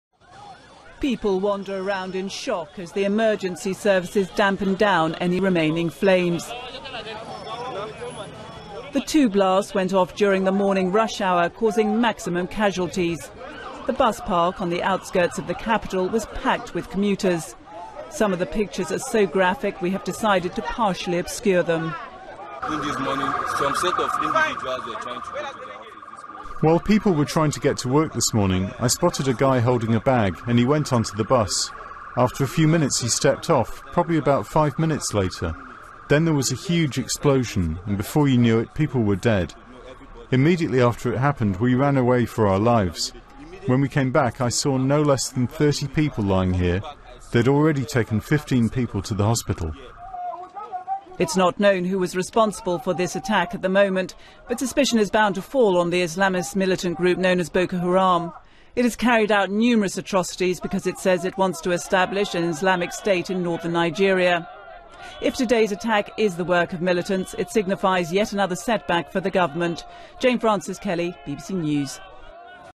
Two explosions kill 'dozens' in Abuja, Nigeria
Dozens of people have been killed in two blasts that rocked a crowded bus station on the outskirts of Nigeria's capital, Abuja, officials say.

The blast happened during morning rush hour as commuters were about to board buses and taxis to go to work.

Eyewitnesses said there were dead bodies scattered around the area.

No-one has claimed responsibility, but the Islamist militant group Boko Haram has carried out attacks in the capital before.

Jane-Frances Kelly reports.